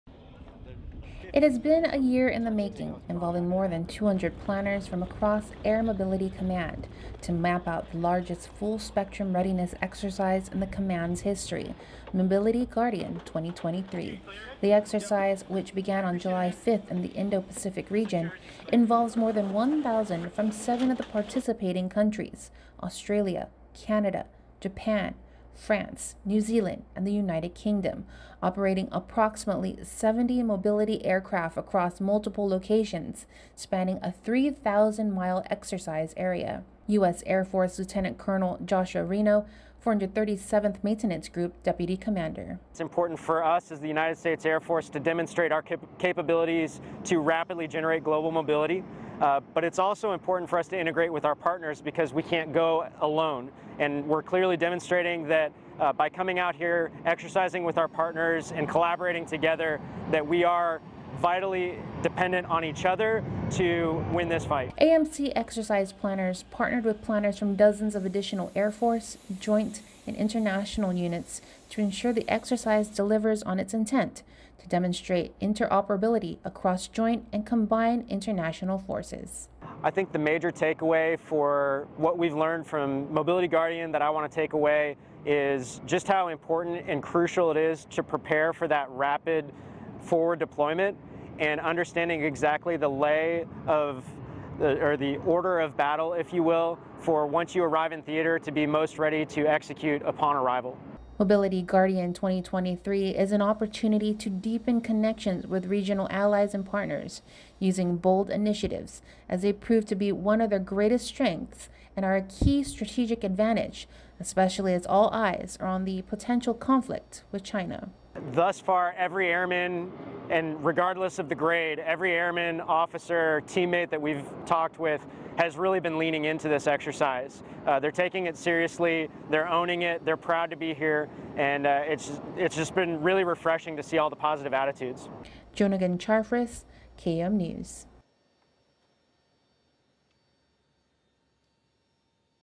Air Mobility Command's largest full-spectrum readiness exercise
Over the past couple of weeks, surely many of you have seen an increase in military presence and flight activity around the island. It's all part of a large-scale exercise: Mobility Guardian 2023, which has seven countries participating.